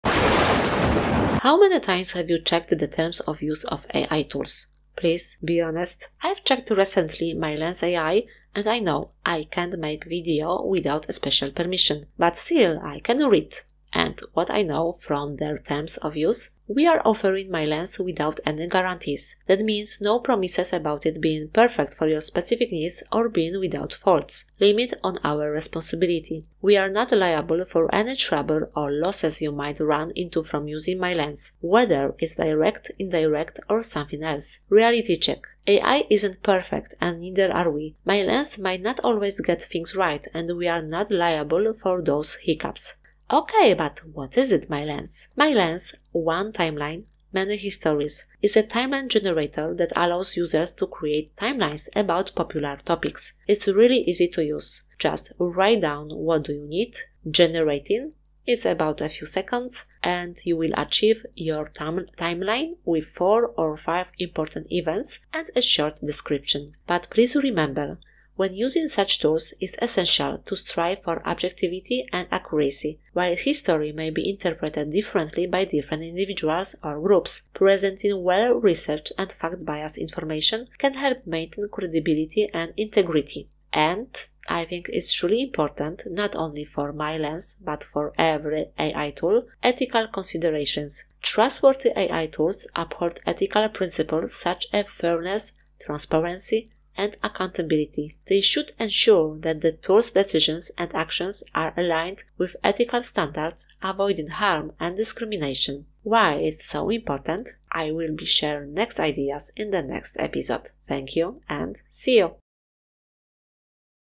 HAVE YOU CHECKED THE TERMS OF USE OF AI TOOLS?
When using such tools, it's essential to strive for objectivity and accuracy. While history may be interpreted differently by different individuals or groups, presenting well-researched and fact-based information can help maintain credibility and integrity.